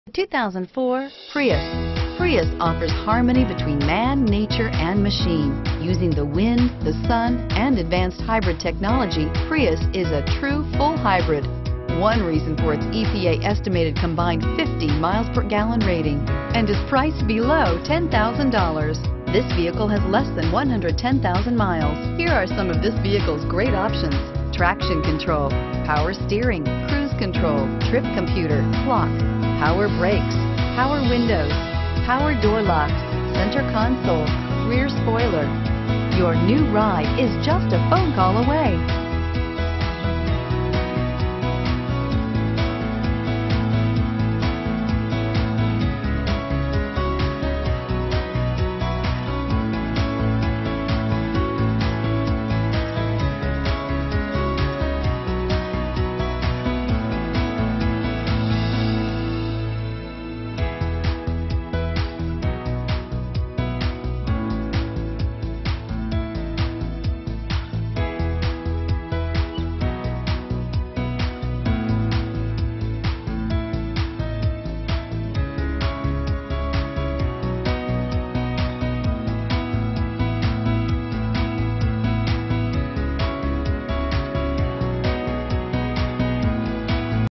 2004 Toyota Prius Schaumburg IL 160884A
2004 Toyota Prius

Schaumburg Toyota
125 W Higgins Rd

You've never felt safer than when you cruise with anti-lock brakes and traction control in this 2004 Toyota Prius.  It comes with a 1.5 liter 4 Cylinder engine.  Don't skimp on safety. Rest easy with a 4 out of 5 star crash test rating.  The exterior is silver.  Climate control lets you set your desired temperature and maintains it while you're in the car.  Spoiler alert! This liftback has a rear spoiler, so you can look and feel like an all-star.  According to a review from New Car Test Drive, All told, the 2004 Toyota Prius is an impressive technological statement, and a car that's easy to like and live with.  Could this be the vehicle for you? Give us a call today and find out!